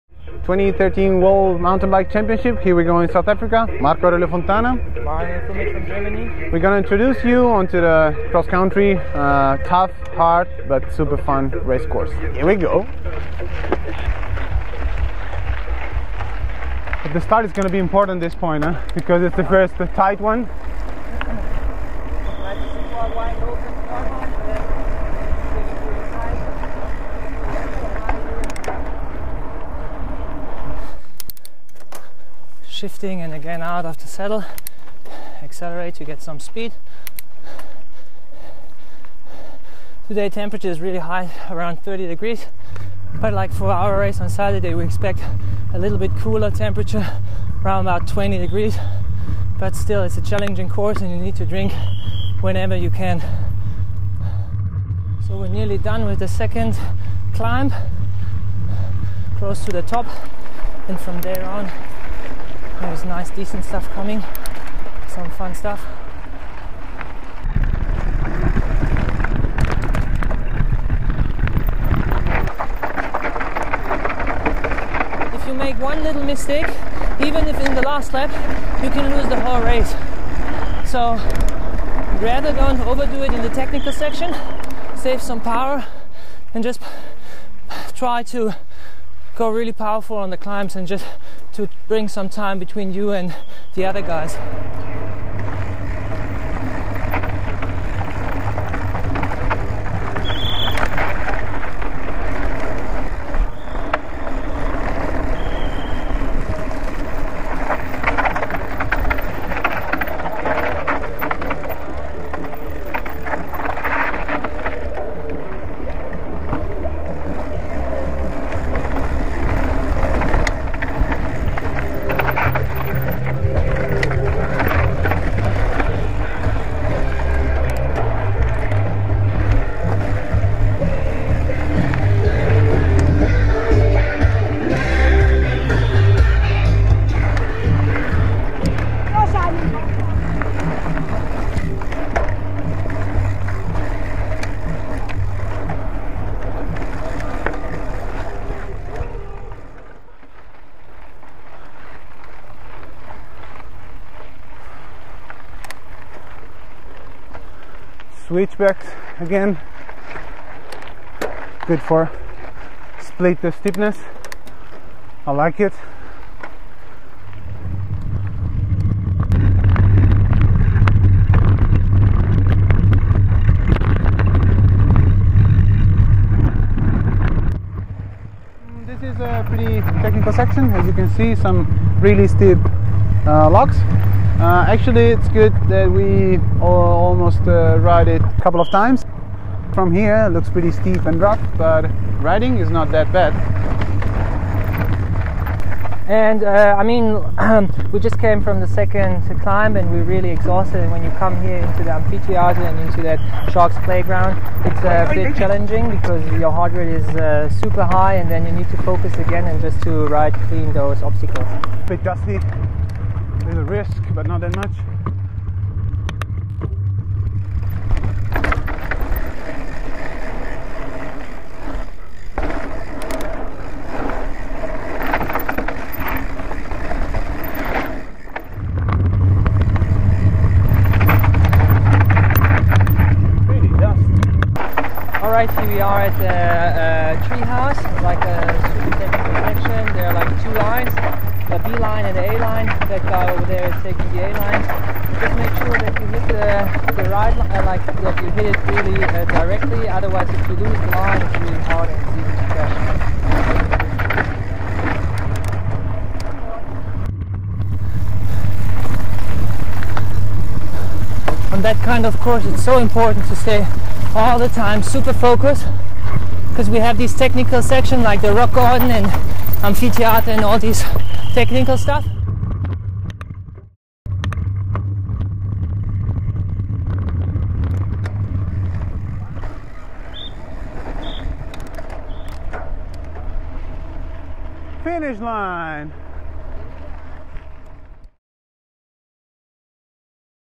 GoPro: UCI MTB World Champs Cross Country Preview
Shot 100% on the HD HERO3 by GoPro, available in South Africa